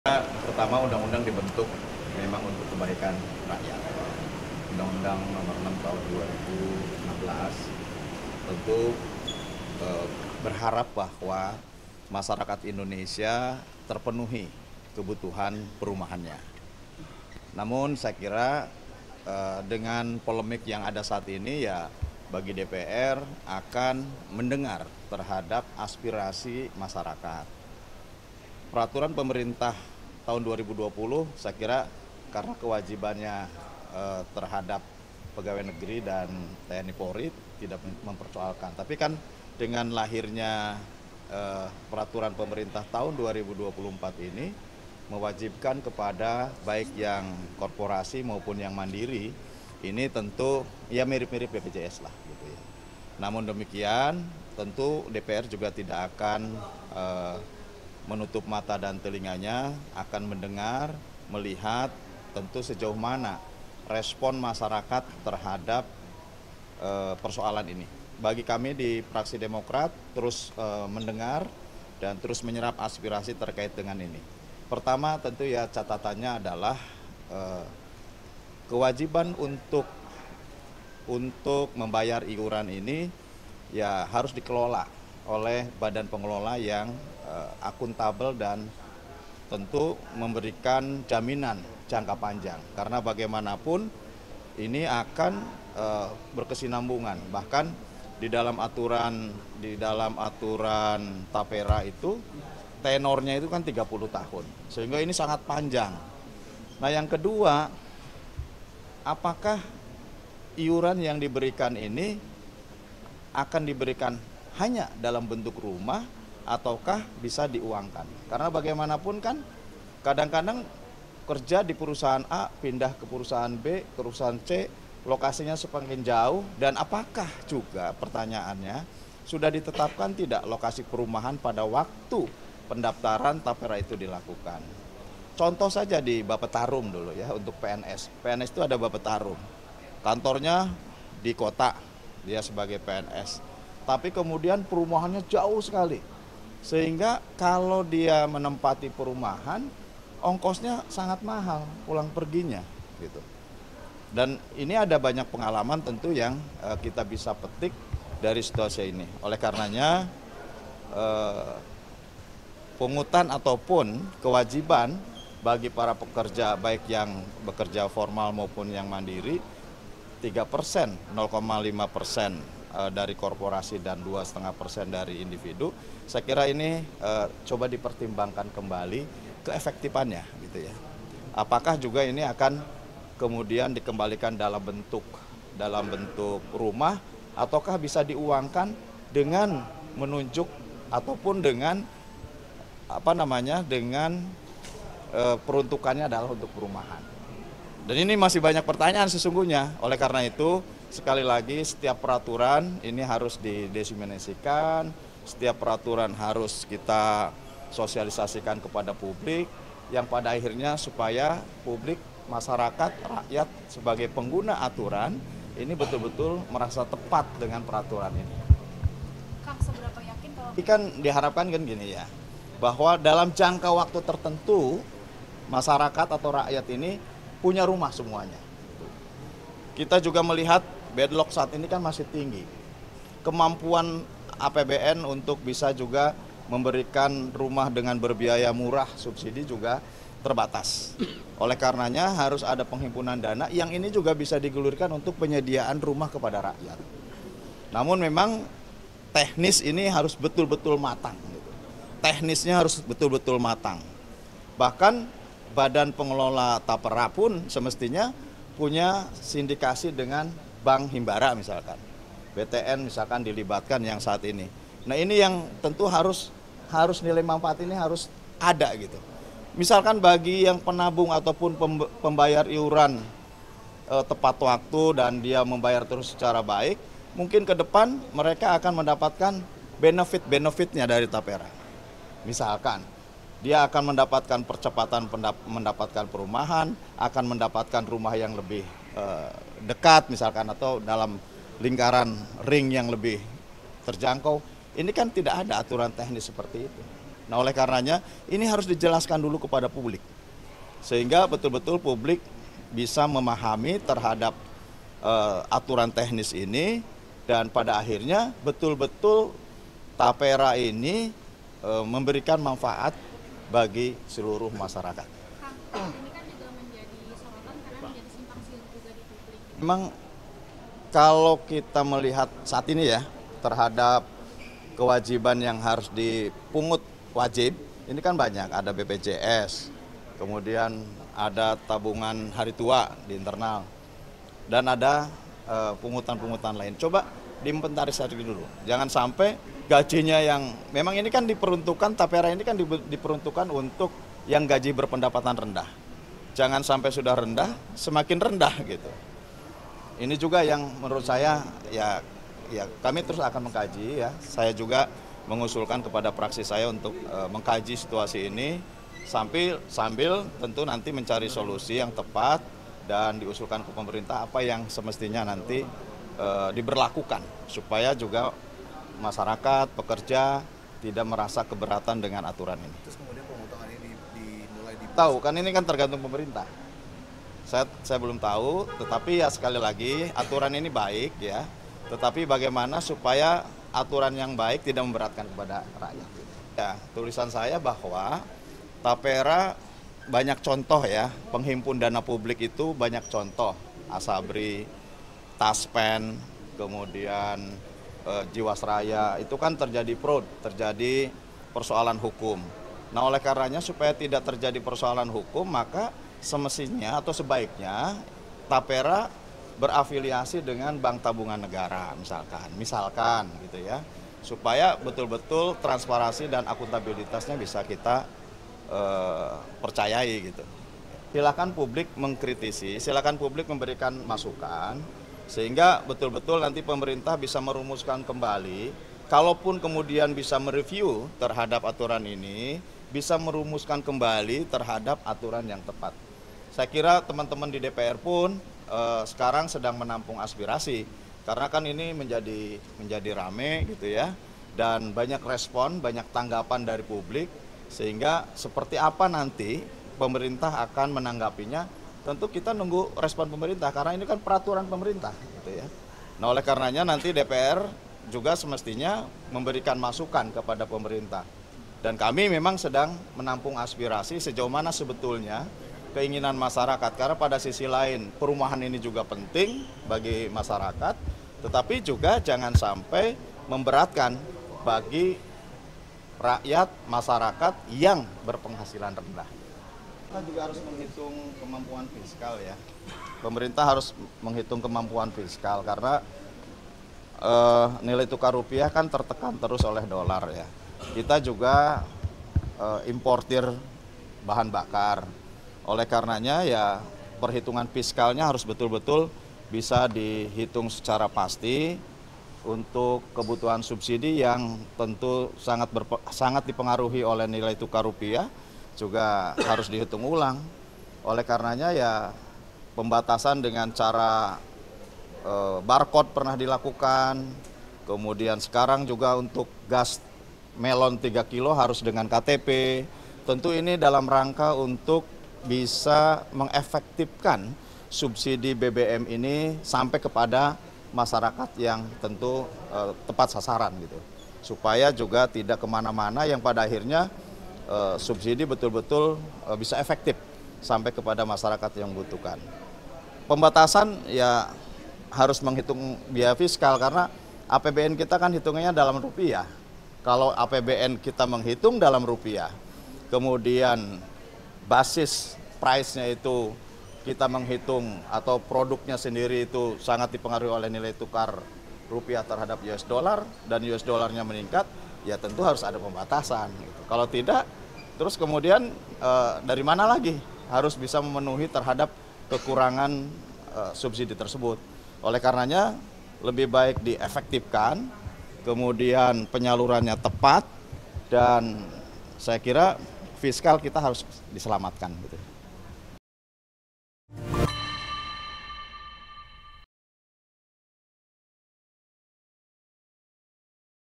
Banyak Orang Tolak Tapera, DPR Jamin Dengarkan Aspirasi Rakyat
Anggota DPR RI dari fraksi Partai Demokrat, Herman Khaeron mengatakan pihaknya akan mendengar dan menyerap aspirasi masyarakat terkait polemik pemotongan gaji untuk iuran Tapera.

Namun ia juga menjelaskan bahwa program Tapera dibuat untuk kepentingan dan kebaikan rakyat, dengan harapan kebutuhan perumahan masyarakat Indonesia dapat terpenuhi.

"Tentu DPR tidak akan menutup mata dan telinga, akan mendengar, melihat tentu sejauh mana respons masyarakat terhadap persoalan ini. Bagi kami di Fraksi Demokrat, terus mendengar dan terus menyerap aspirasi," kata Herman.

"Nah oleh karenanya nanti DPR juga semestinya memberikan masukan kepada pemerintah dan kami memang sedang menampung aspirasi sejauh mana sebetulnya keinginan masyarakat," lanjutnya.